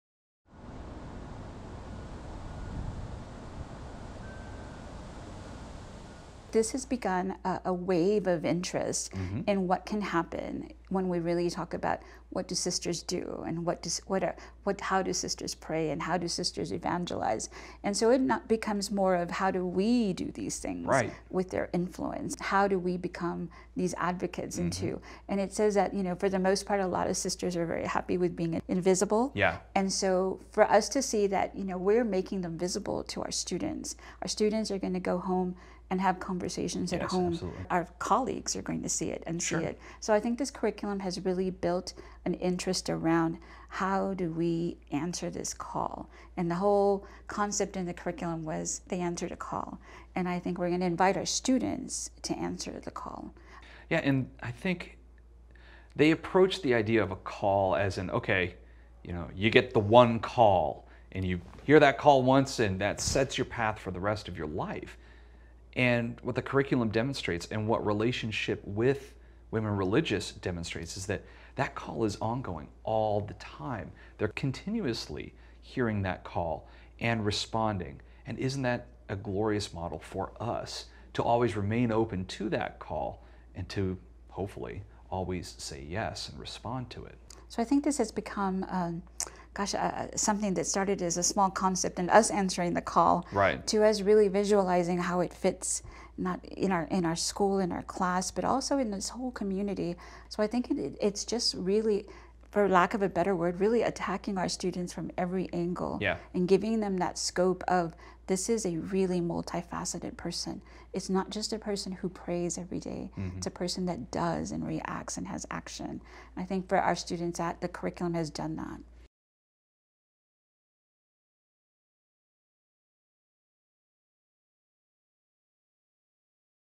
Called and Consecrated: How To Use The Curriculum
Dr. Chris Samuel (religion teacher at St. Anthony’s Catholic HS in San Antonio) and Rosi Cortez (Director of Mission and Ministry at St. Anthony’s Catholic HS and Incarnate Word HS in San Antonio) discuss how to use the NCSP curriculum and how students can benefit from learning about sisters and their lives.